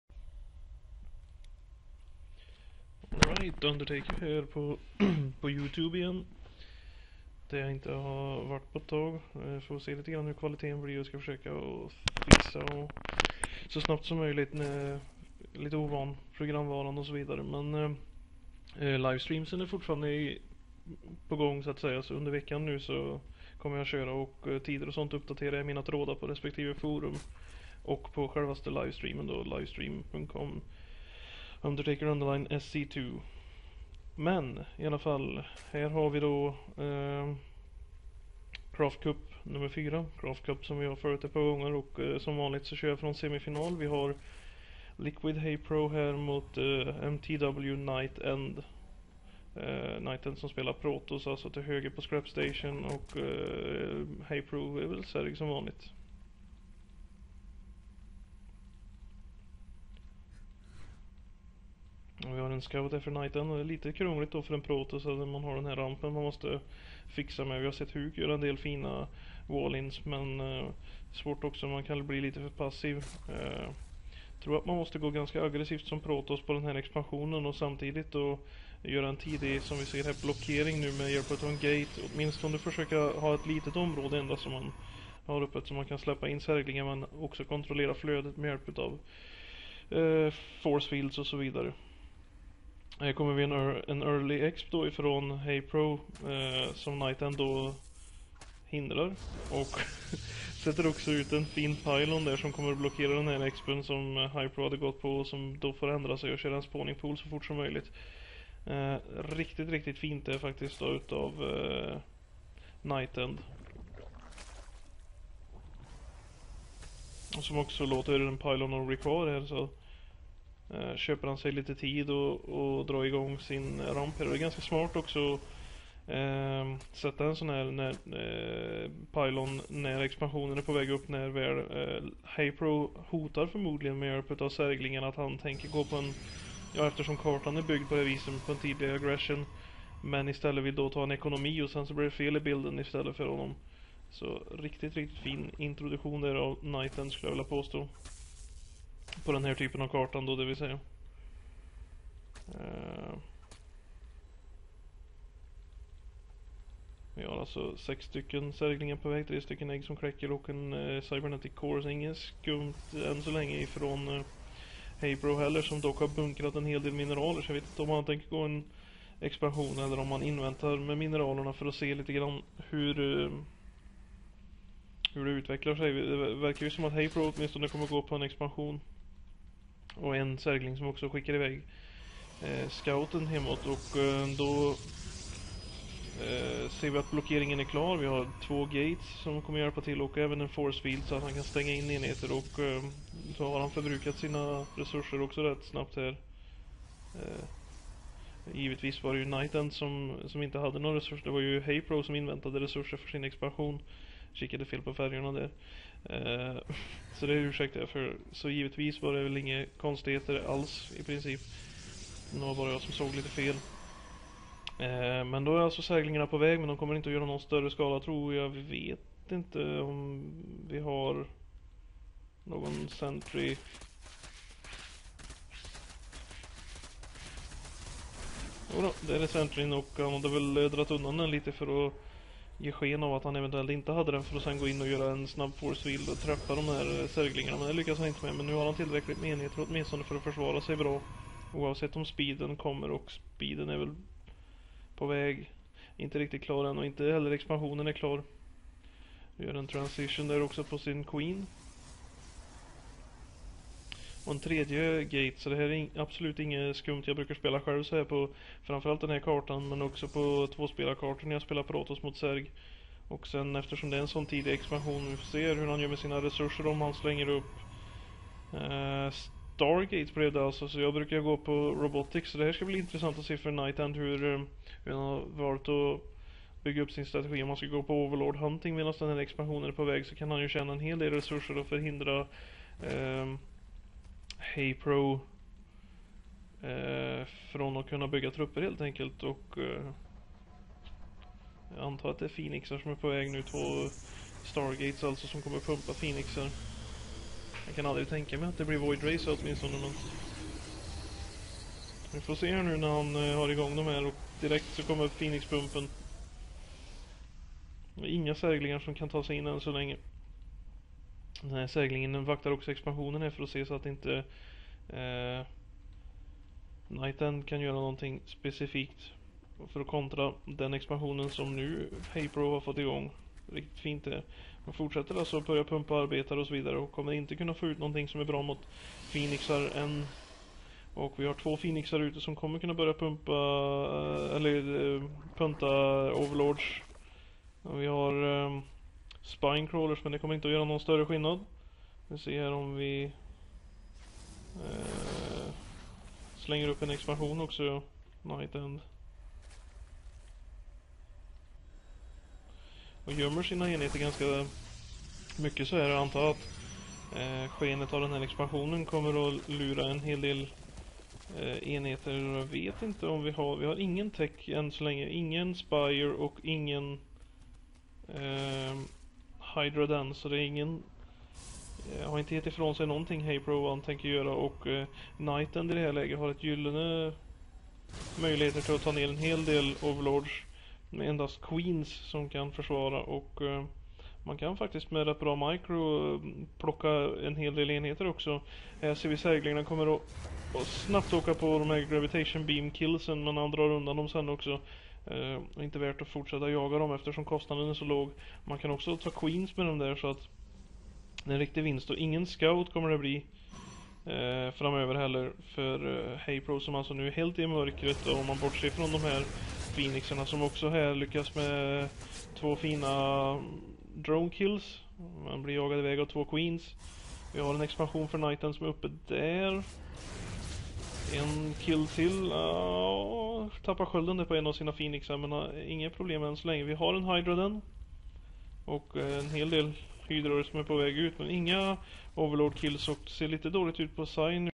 NightEnd vs HayPro.mp4
Game 1 semi-final Nightend vs Haypro part 1 / 2

Commentary : Swedish